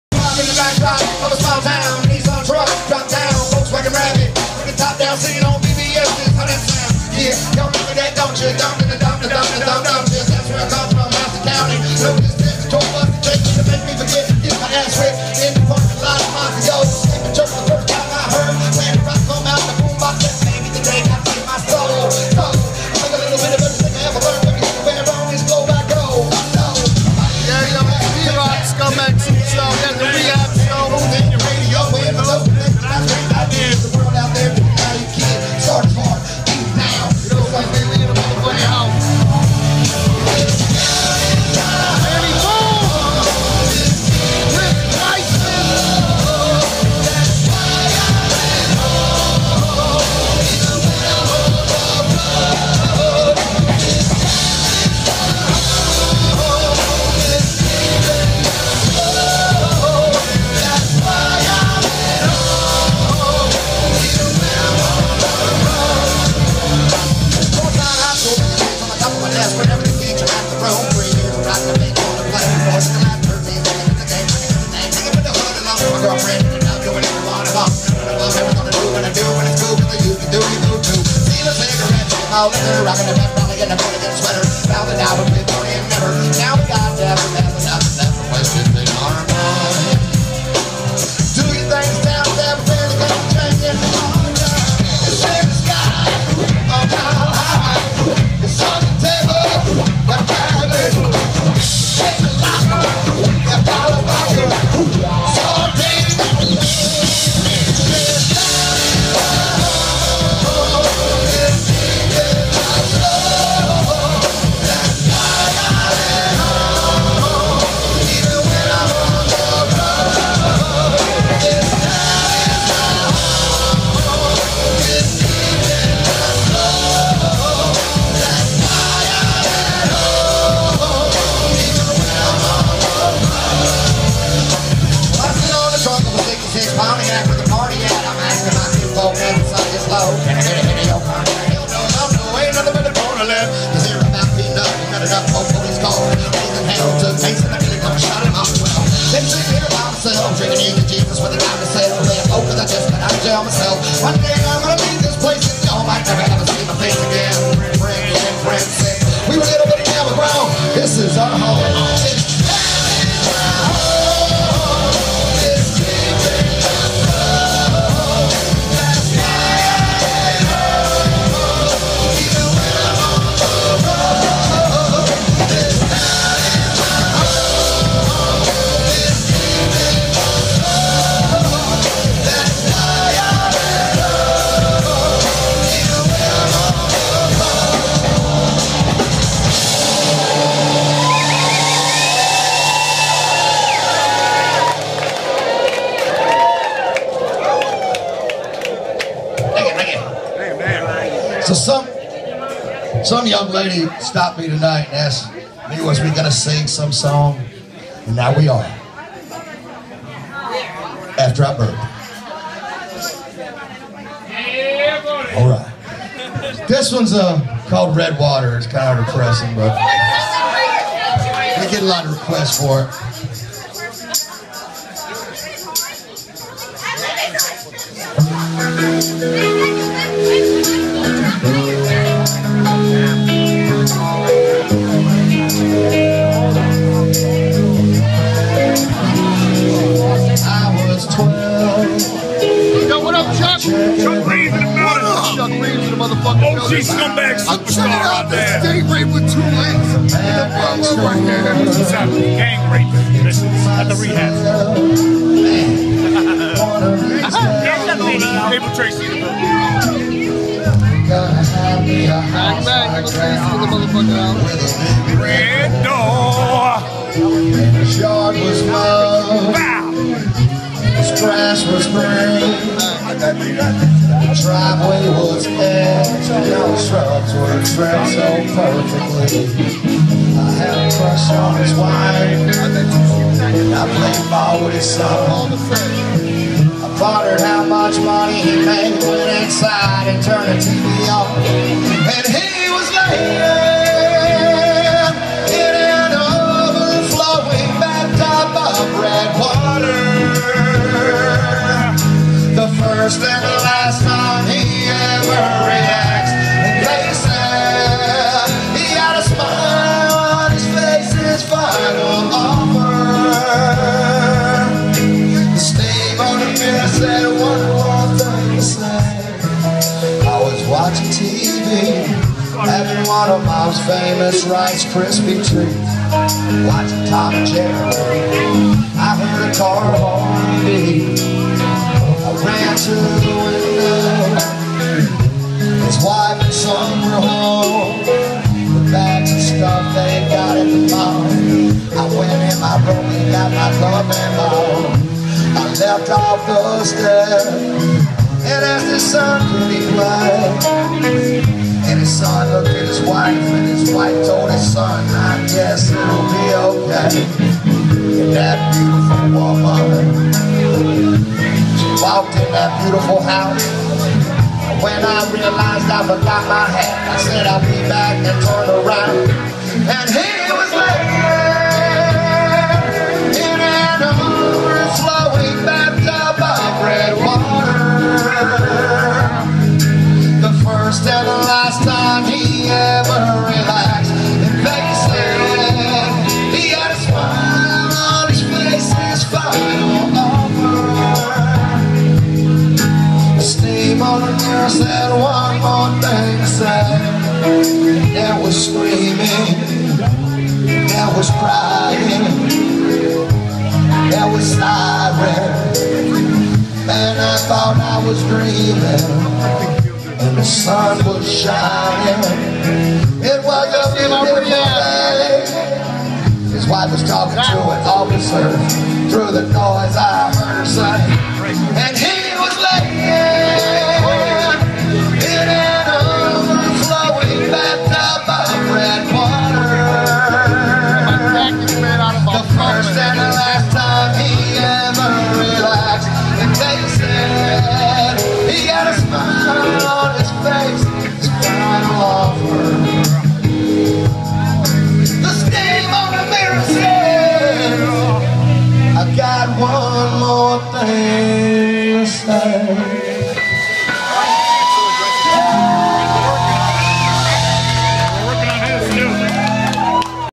Rehab Final Tour Providence RI 3/6/2014
Rehab Final Show in Providence Rhode Island at Simons667 filmed by D.Rocc from ScumbagSuperstar Hosted by Chuck Reaves